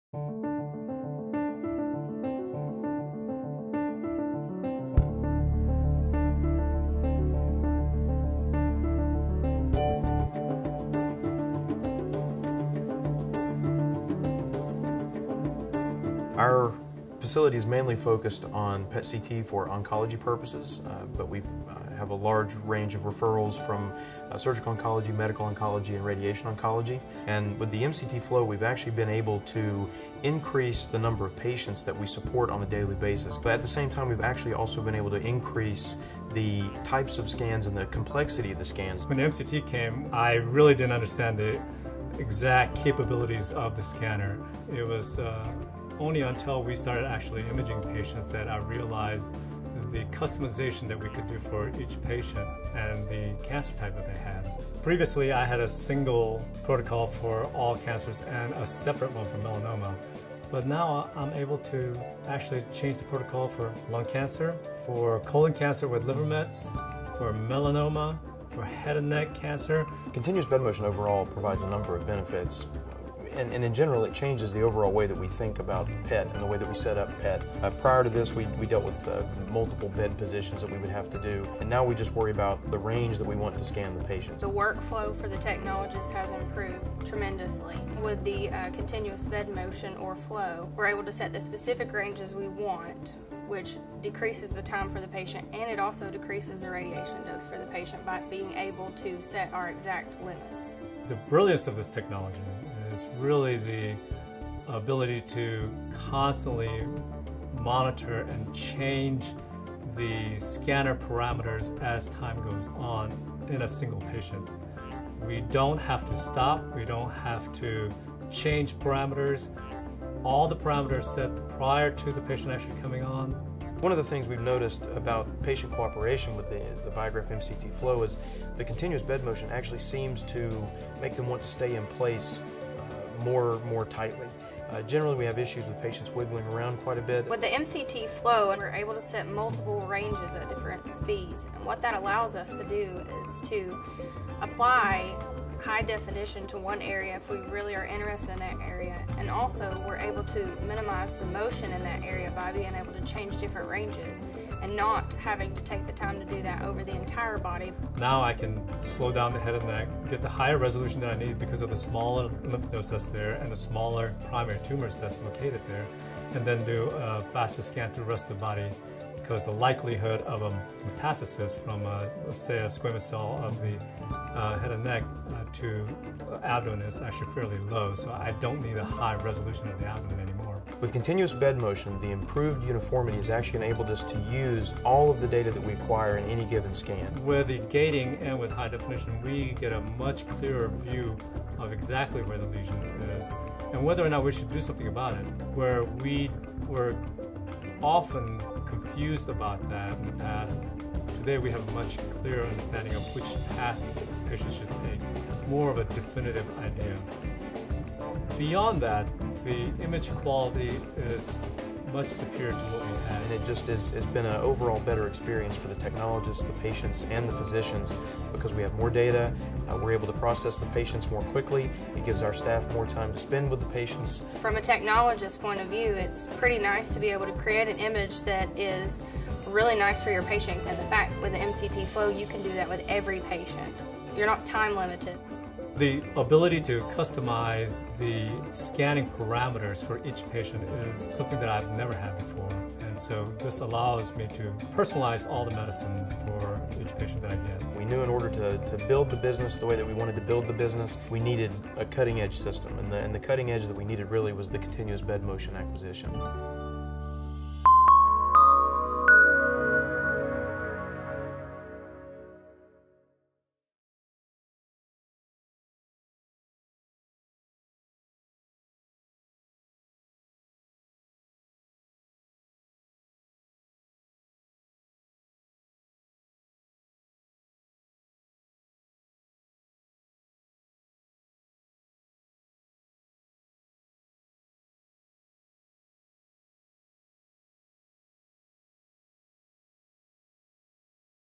PET/CT - Achieving Workflow & Clinical Improvements
The University of Tennessee Medical Center is one of the first medical research facilities in the country to experience the benefits of Siemens Biograph mCT Flow™ to better support its PET/CT imaging for its patients. As a result, the hospital has been able to increase the efficiency and throughput of its PET/CT exams, while simultaneously increasing the complexity of the exams they can perform.